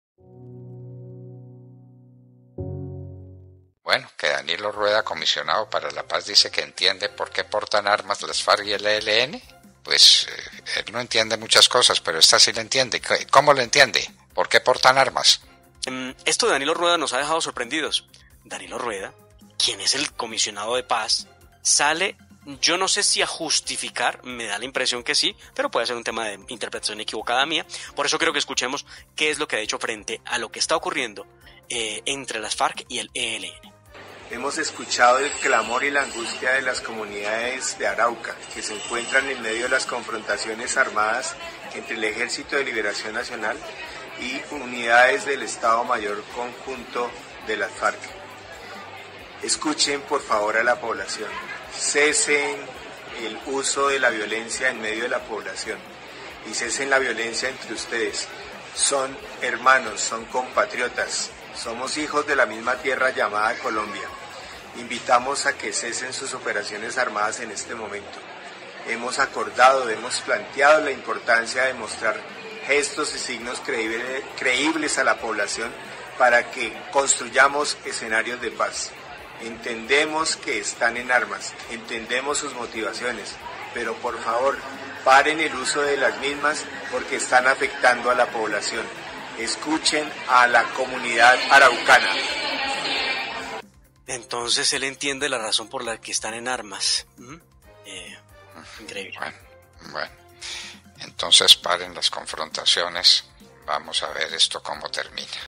LOÚLTIMO ⭕ SORPRENDEN LAS ECLARACIONES DE DANILO RUEDA
Alto comisionado de Paz pidió al Eln y a disidencias de las Farc detener las confrontaciones que sostienen en Arauca